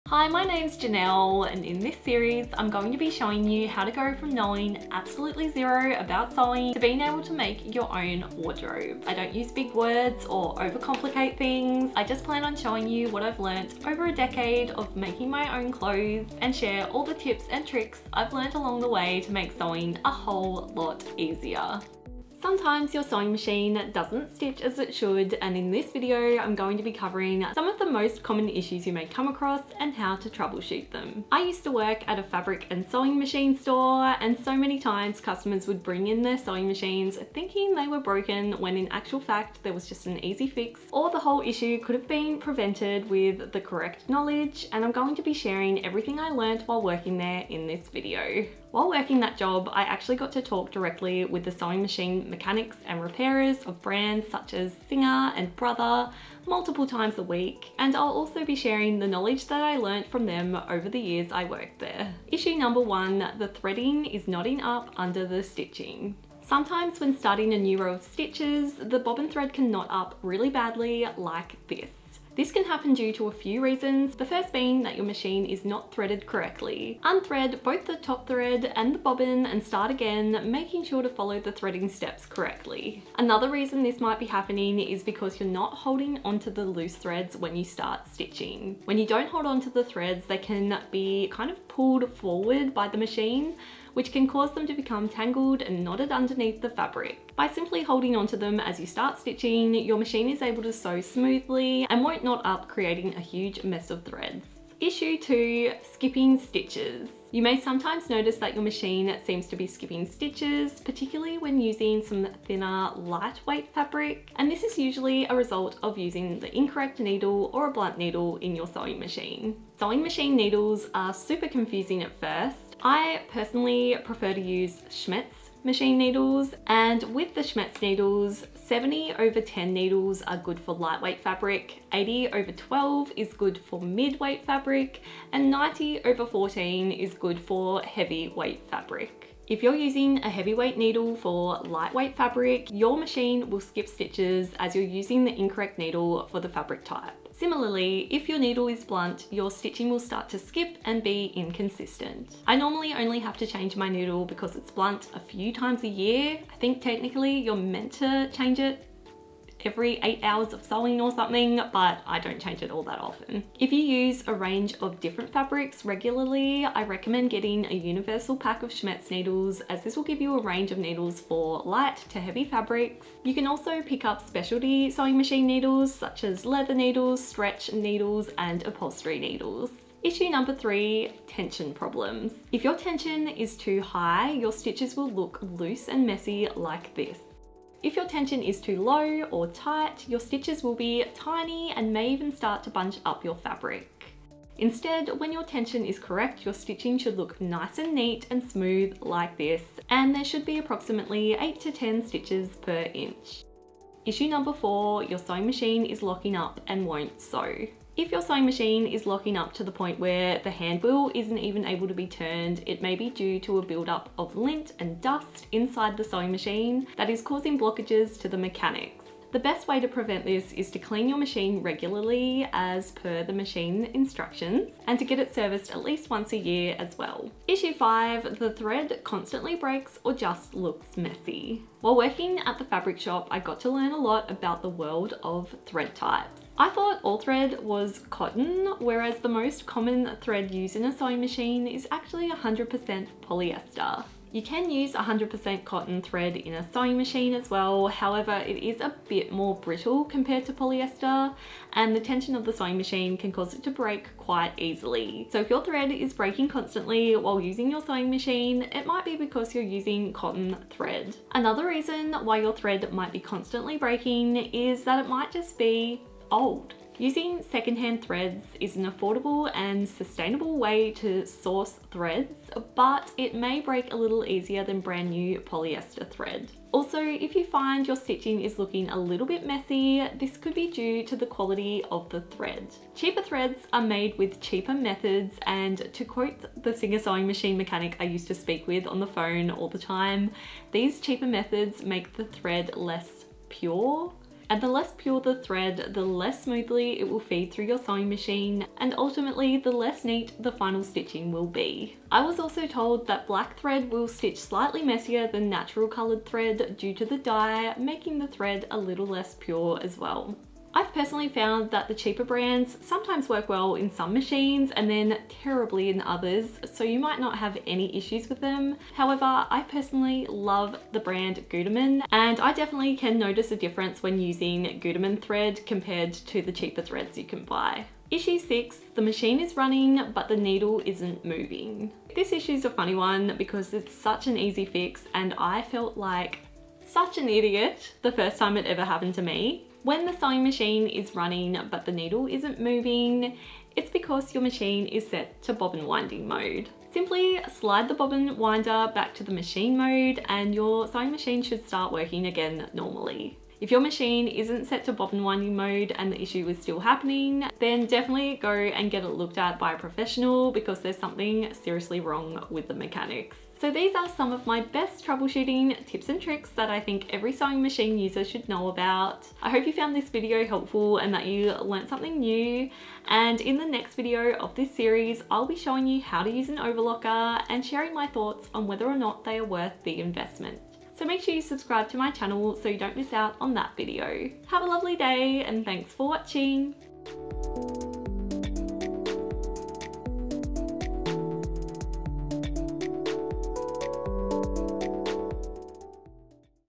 Sewing Machine Troubleshooting Tips (When Things Don't Work As They Should) | Sewing For Beginners
Sewing for beginners - episode 4: Sewing machine troubleshooting tips and tricks! For when things don't work exactly as they should.

Chapters:

00:00 Intro
01:02 Thread is knotting up underneath stitches
01:45 Sewing machine is skipping stitches
03:00 Tension problems
03:25 Sewing machine is locking up and won't sew
03:48 Sewing machine thread constantly breaks
05:29 Sewing machine needle isn't moving
06:03 Thanks for watching!

Things mentioned:

Schmetz sewing machine needles
Guttermann thread

Equipment Used:

Camera: Fujifilm X-T4
Sewing machine: Brother FS101 (gifted)
Overlocker/Serger: Singer 14sh644